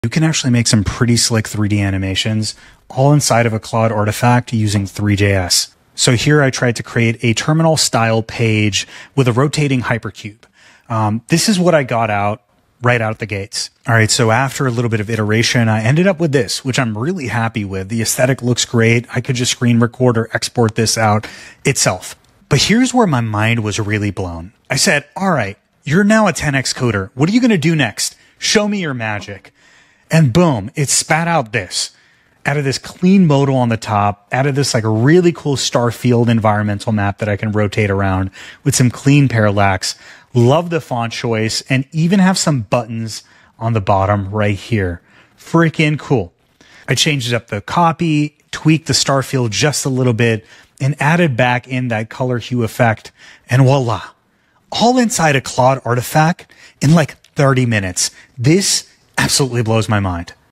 Claude 3.5 Builds Stunning 3D Website Using Three.js
Quick tip: Use Claude Artifacts with Three.js to create 3D graphics on the web. I started with a basic hypercube animation. Then I challenged Claude to be a '10x coder'. And to my surprise, it delivered an extremely elevated redesign! Pretty wild. You can do in 30 minutes what previously took a day of work. Now if only Claude had a higher rate limit!